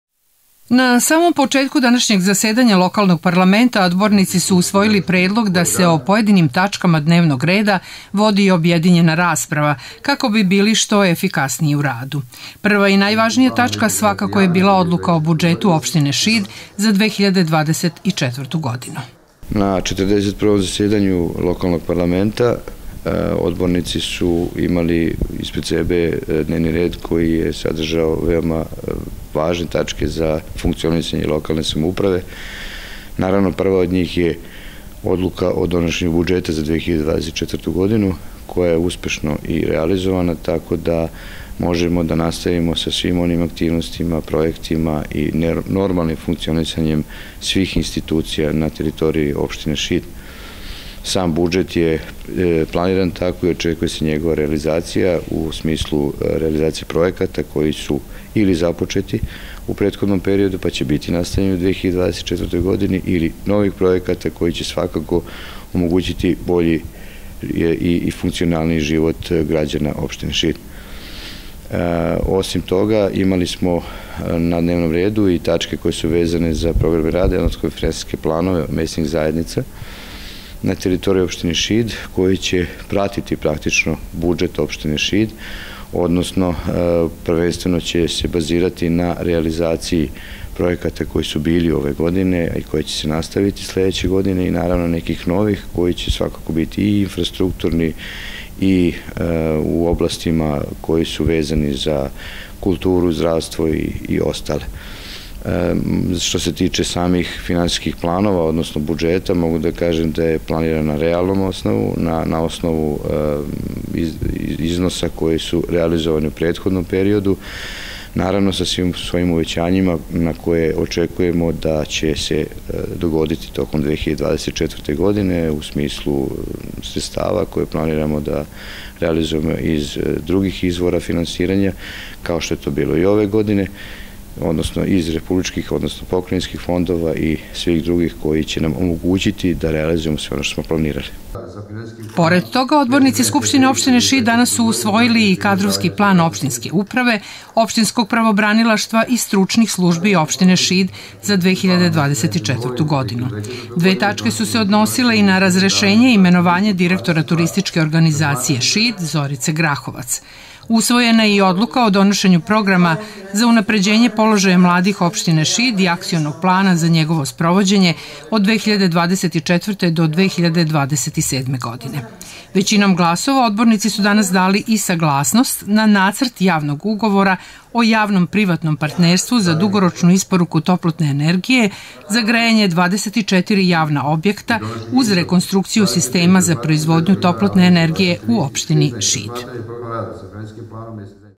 Sednica SO Šid
U sali Kulturno obrazovnog centra Šid danas je održana 41. sednica Skupštine Opštine Šid. Na dnevnom redu bilo je 26 tačaka, među kojima: Odluka o budžetu za 2024. godinu, programi rada i finansijski planovi za svih 19 mesnih zajednica u opštini Šid i Program za unapređenja položaja mladih.